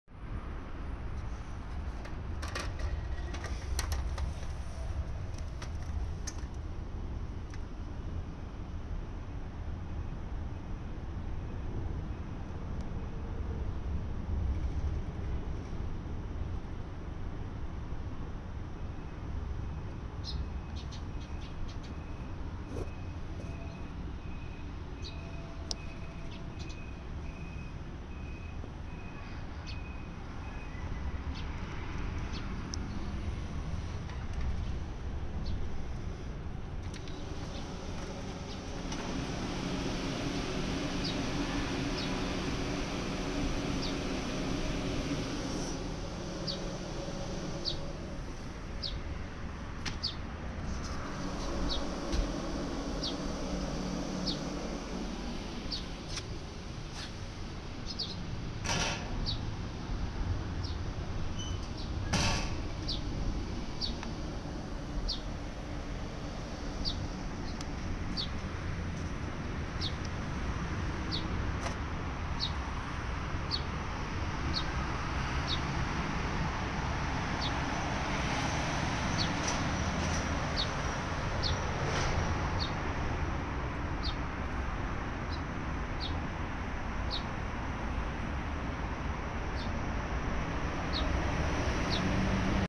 ダイワ 下置型 スライド扉冷蔵ショーケース 「603AUJ-EC」 2017年製 動作確認動画
ダイワ製冷蔵ショーケース「603AUJ-EC」　2017年製です。
冷却動作に問題ありません。
動作音も非常に静かです。
コンビニエンスストアにて使用されていたもので、きれいに使われておられたものになります。